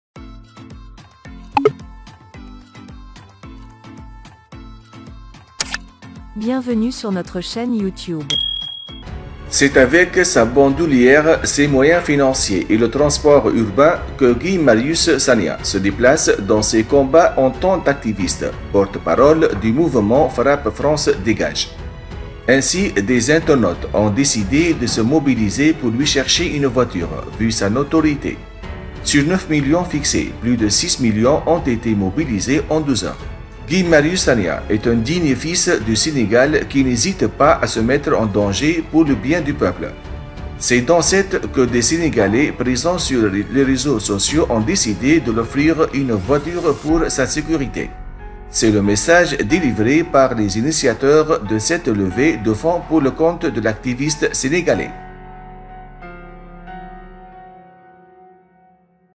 Voiture pour Guy Marius Sagna : Des internautes mobilisent plus de 6 millions en 12 heures
Au  Sénégal, des internautes ont lancé une campagne de levée de fonds, ce samedi 27 mars,  pour trouver une voiture à l’activiste Guy Marius Sagna.  En 12 heures seulement, le montant mobilisé a atteint  6 millions de F Cfa.